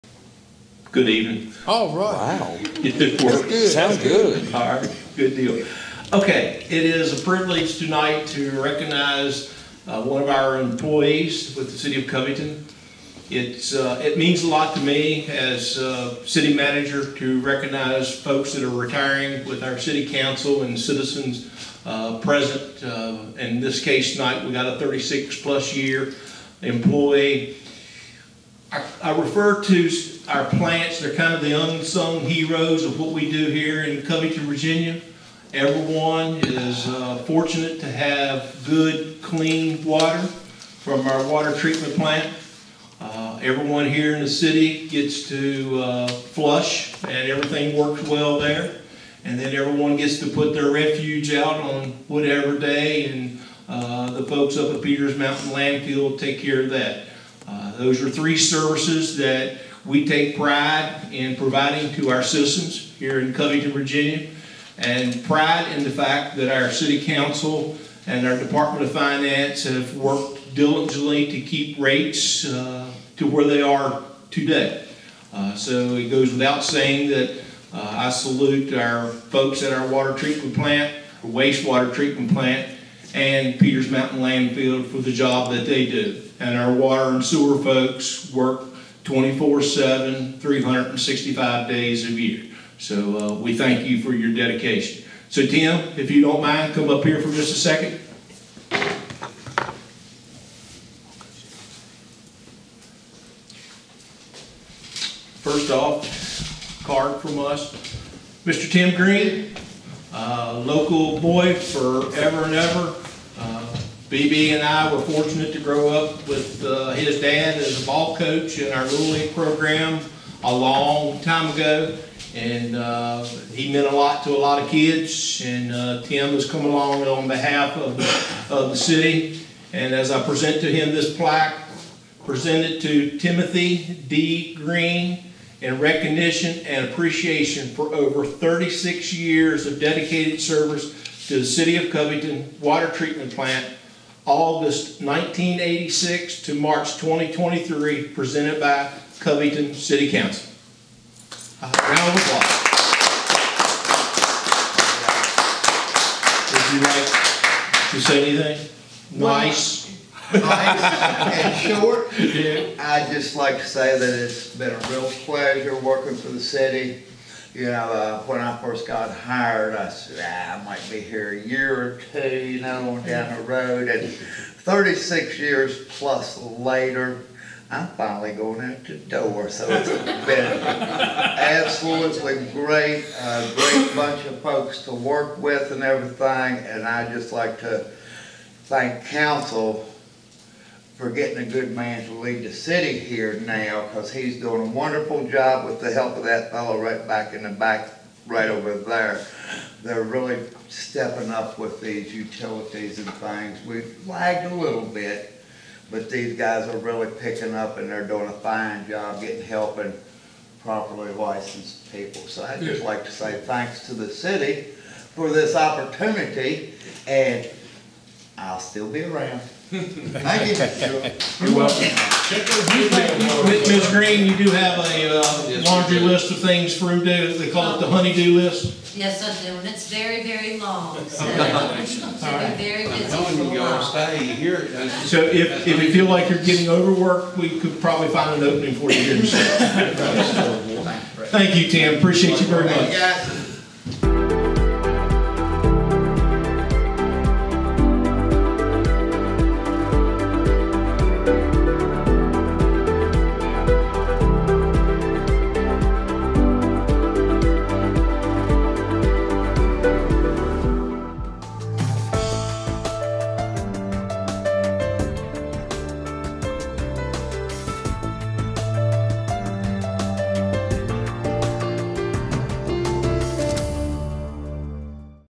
Tim Green Honored For 36 Years Of Service To Covington
Tim Green has spent the last 36 years working for the city of Covington, Virginia. On Tuesday, April 4, 2023, the city and specifically, its city manager Allen Dressler, noted that multi-decades of dedication at a ceremony at city hall.